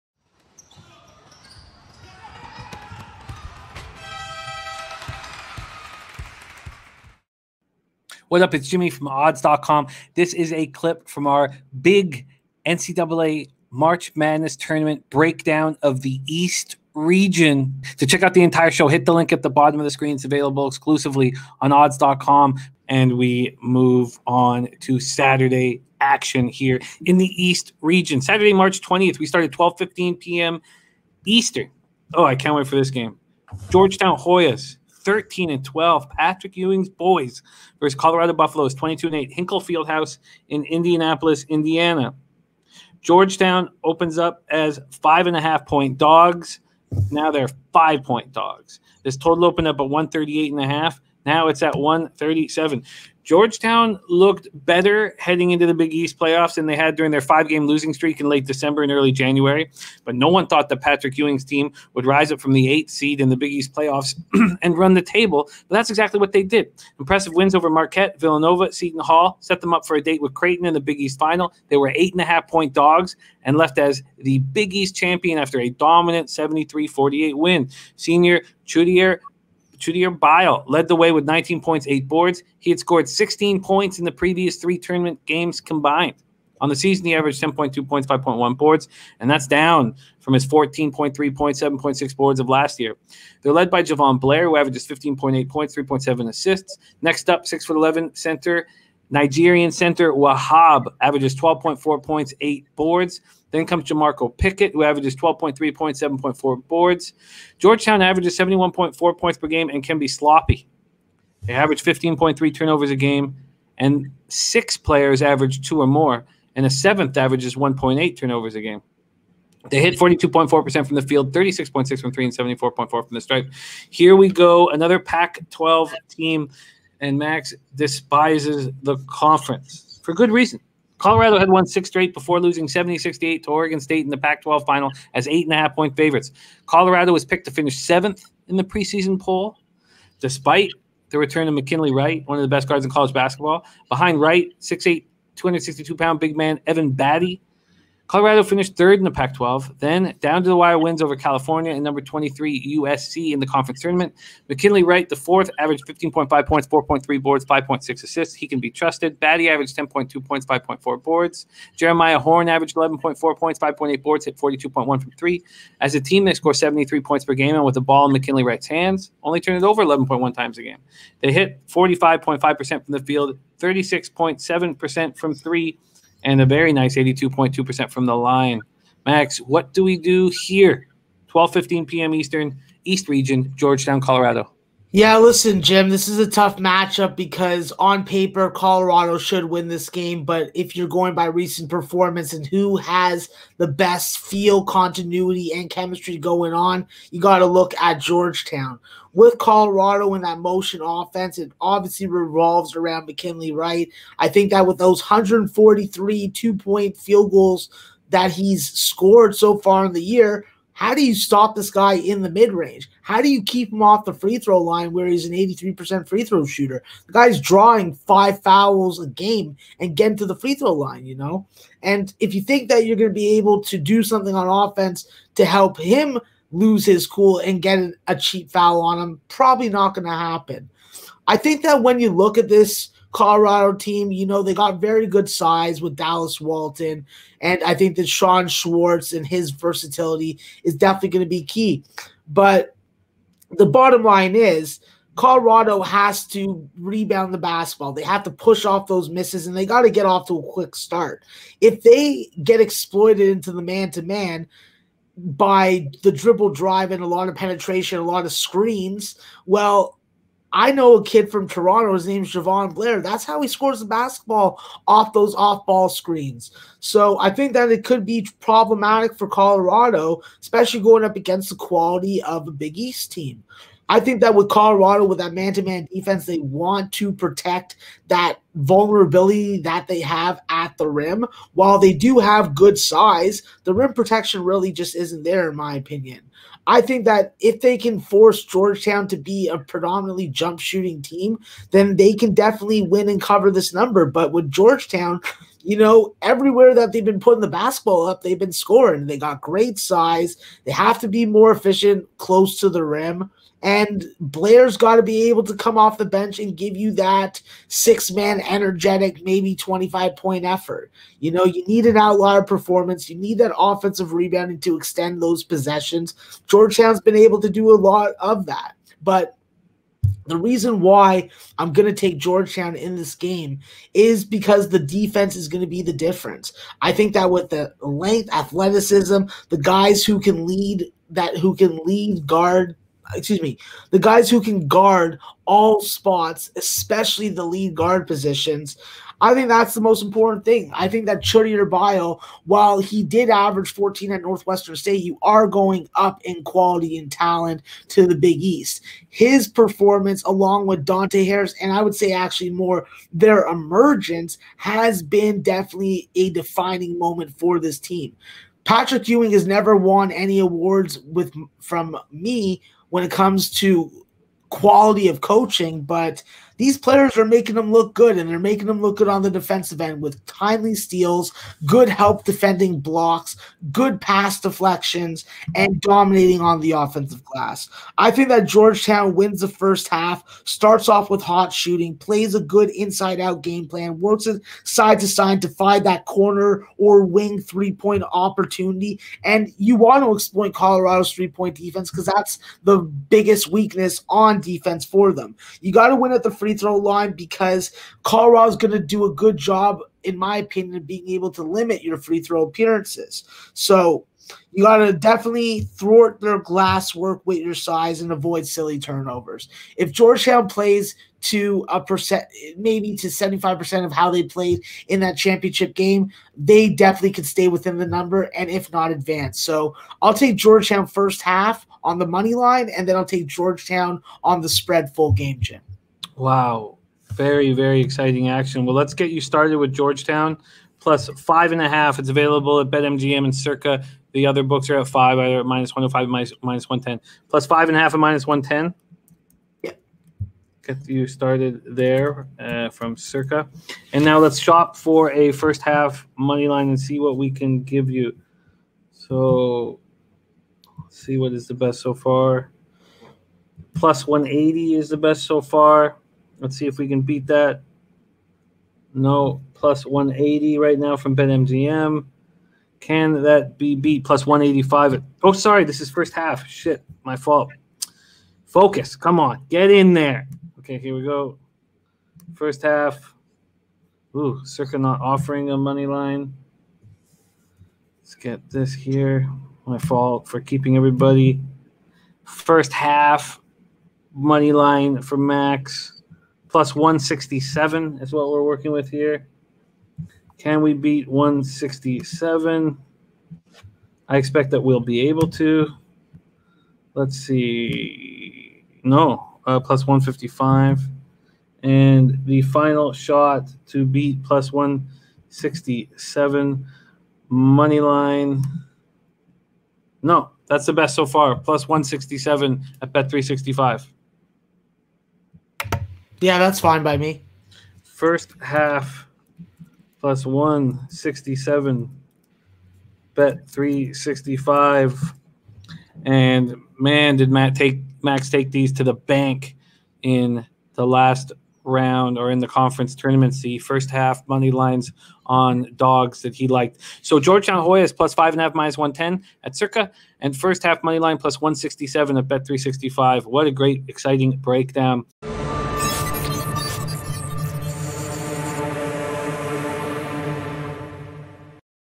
Georgetown vs Colorado Pick | March Madness Odds | East Region - Round 1 Picks
East Region - Round 1
#12 Georgetown vs #5 Colorado

March Madness Odds | East Region - Round 1 Picks | College Basketball Predictions

We're halfway through our coverage of the first round of March Madness, and our next stop is the East region, where the Michigan Wolverines come in as the odds on favorite to come out of this field but with many hurdles to climb, including a second-round match-up with either the Bayou Bengals or the Bonnies. Join Maxwell Smart and Jimmy the Bag as they breakdown all the opening round games from the East region.

In this special clip, Max and Jimmy breakdown this round one matchup. For more March Madness picks, odds and all that good stuff, be sure to stay tunes